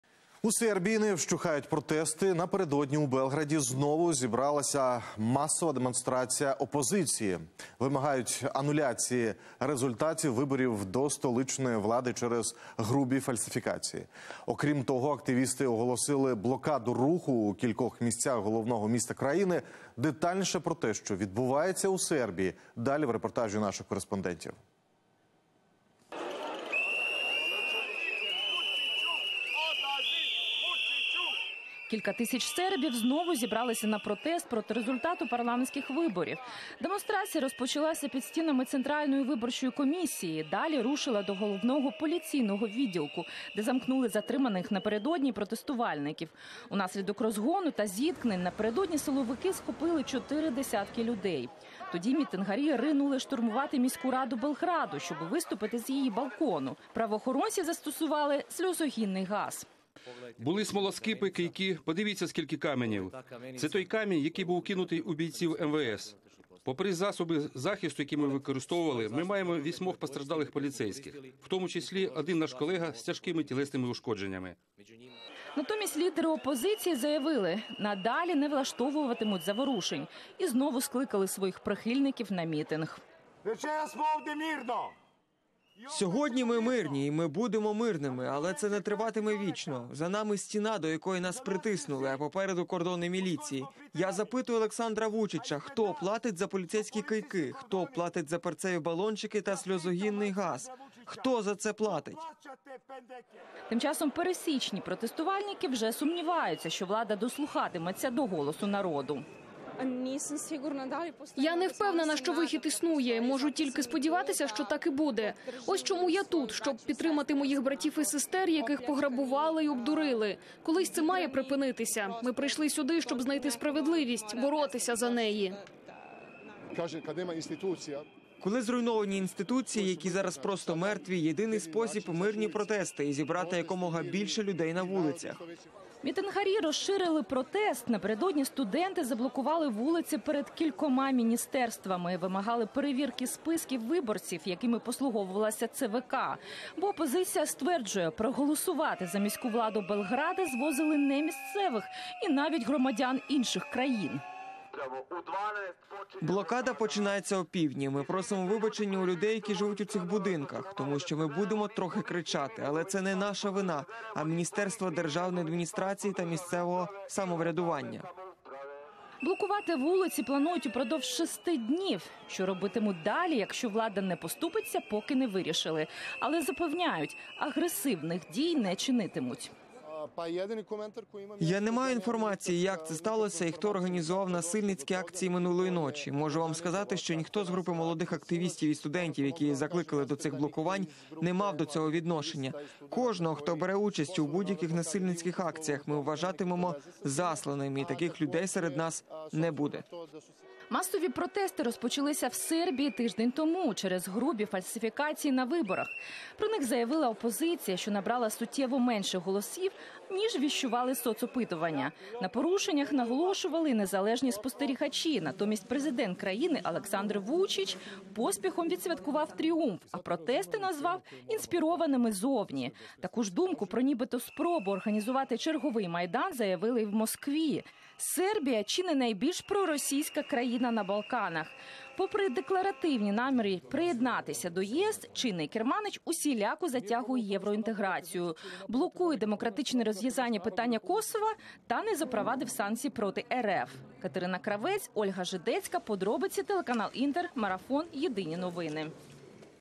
У Сербії не вщухають протести: у Белграді знову зібралася масова демонстрація опозиції
Окрім того, активісти оголосили блокаду руху у кількох місцях головного міста країни. Докладніше про те, що відбувається в Сербії, далі в репортажі наших кореспондетів.